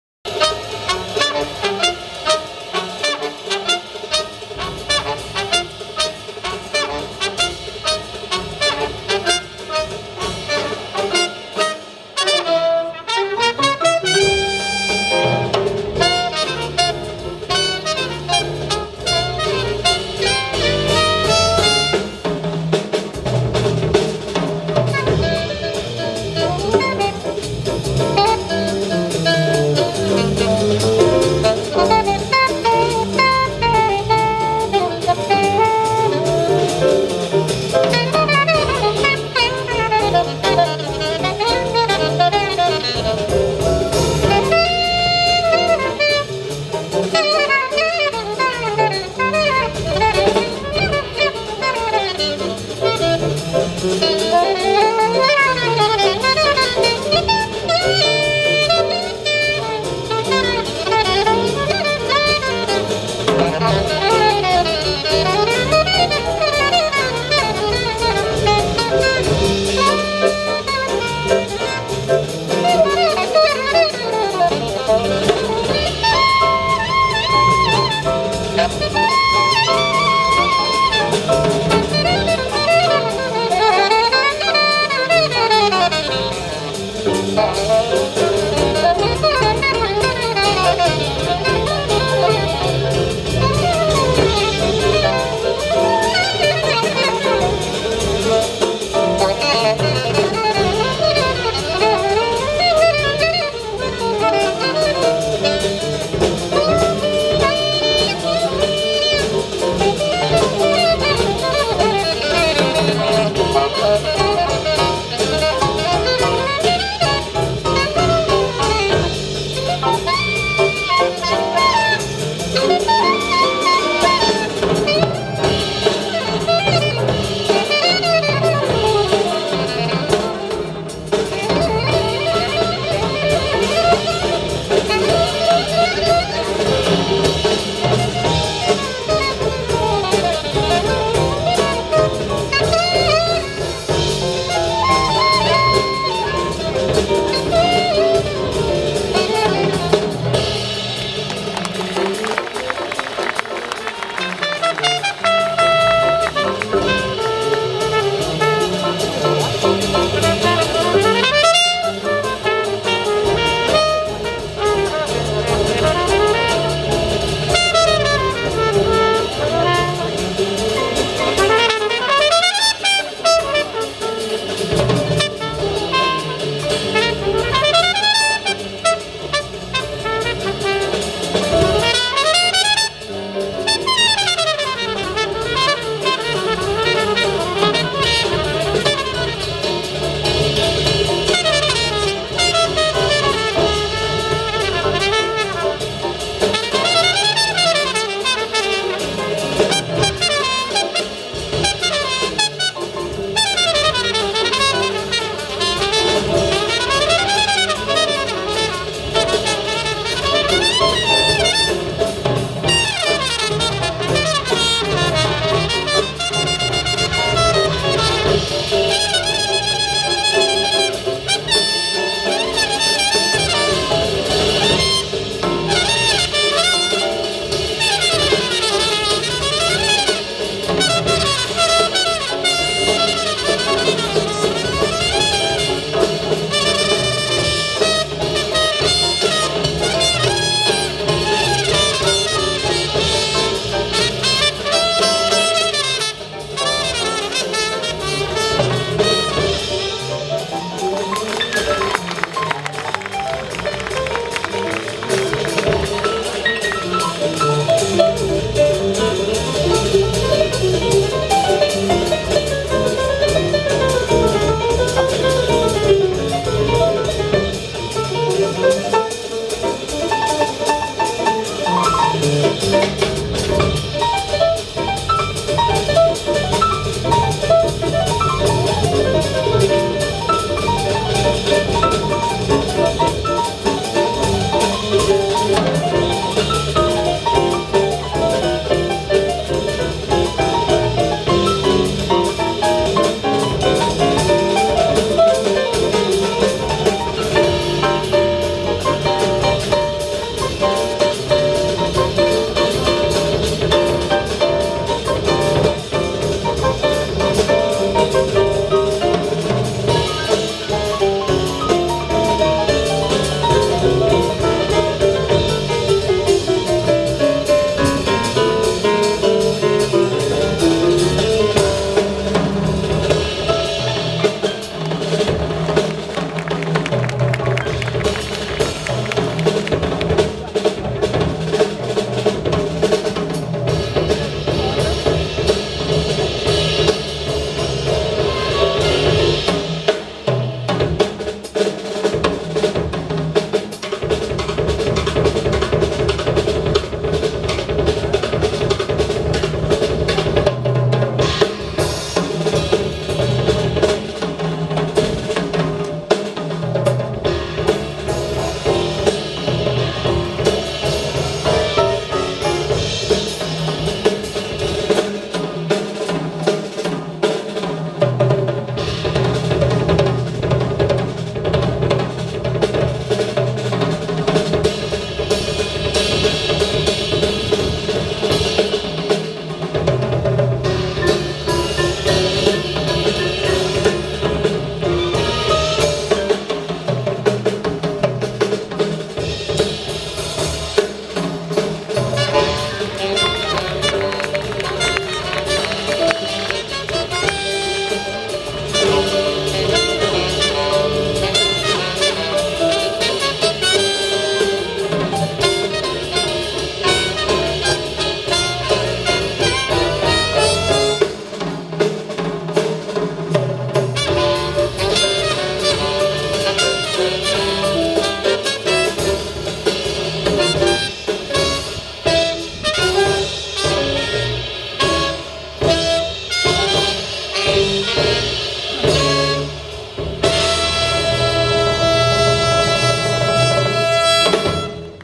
Sea of JazzFest Concert at the Amp!
Sea of JazzFest with great talents Terell Stafford on Trumpet,  Dick Oatts on Saxophone, David Finck on Bass and Emmet Cohen on Piano.